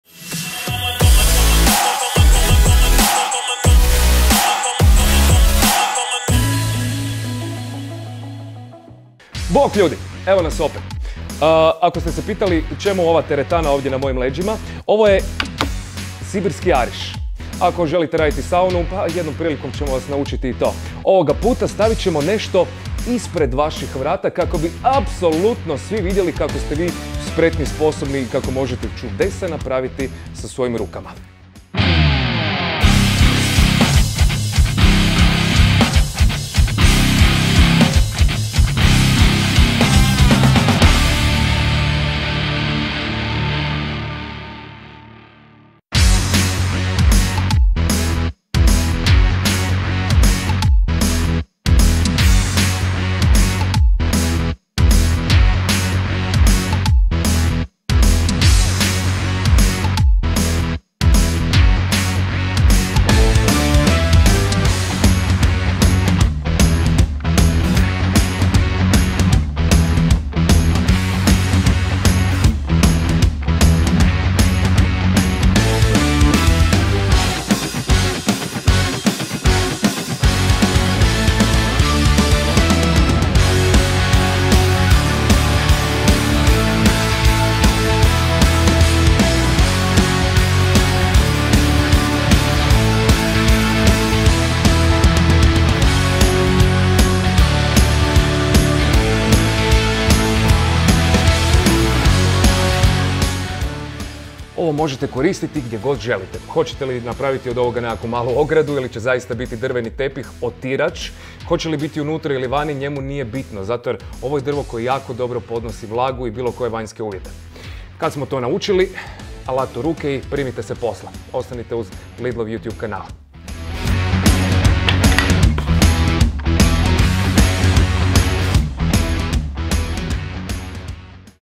Nauči nešto vješto - Drveni tepih - Parkside by Daniel Bilić
Lidl je za vas pripremio novi serijal - Nauči nešto vješto u kojem vam poznate osobe donose savjete i trikove kako naučiti nešto vješto i postati pravi majstor zanata!

U ovom serijalu poznati glas i stats hrvatskih ekrana i radio postaja - Daniel Bilić - donosi vam savjete kako Parkside alatom načiniti razne korisne i ukrasne predmete.

U ovom videu ćemo pokazati kako pomoću Parkside alata napraviti drveni tepih!